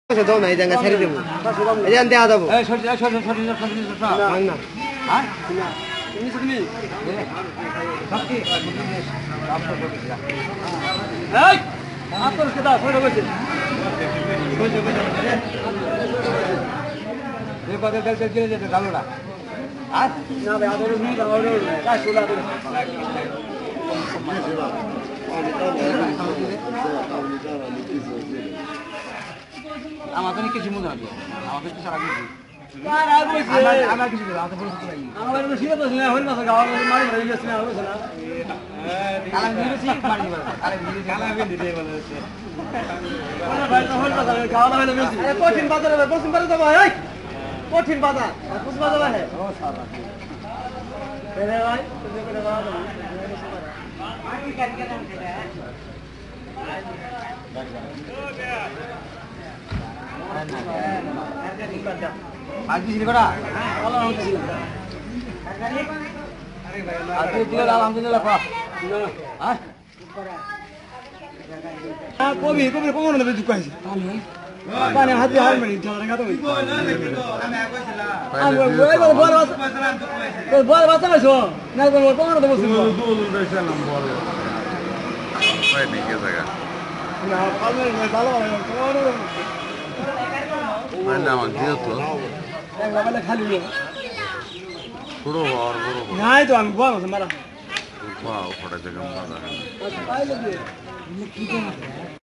Shoal fish baby fruit, in the village market!
গ্রাম্য বাজারে শৌল মাছের বাচ্চা লাফালাফি, দেখে নির্মল বিনোদন পাবেন।Shoal fish baby fruit, in the village market.Shoal fish baby fruit, in the village market.